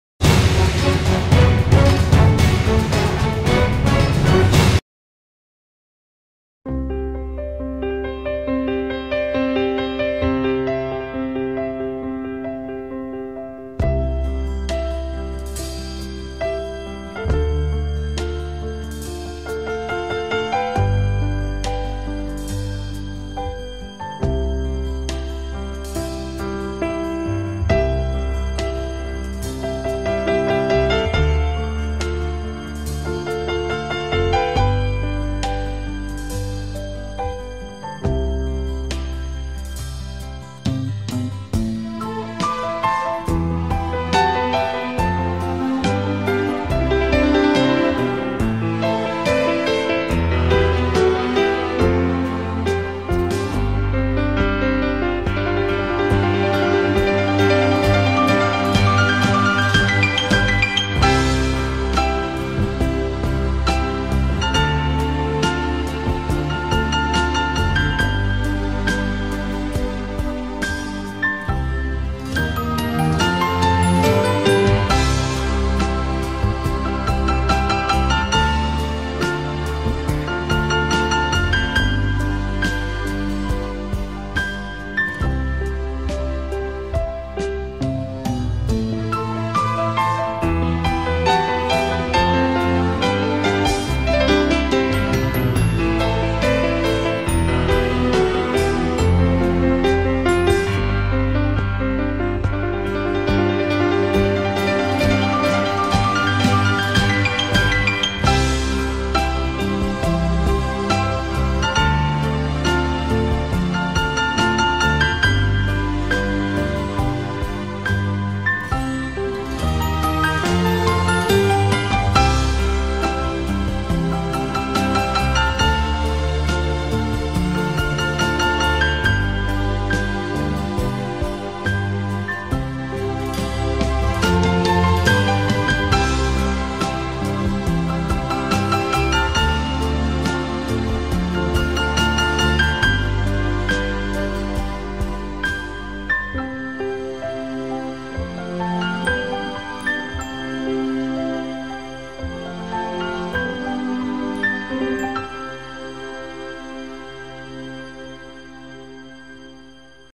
Visiter le Vietnam en images - HORIZON VIETNAM TRAVEL
Visiter le Vietnam ; pays de beautés naturelles fascinantes, de beaux sourires et de convivialité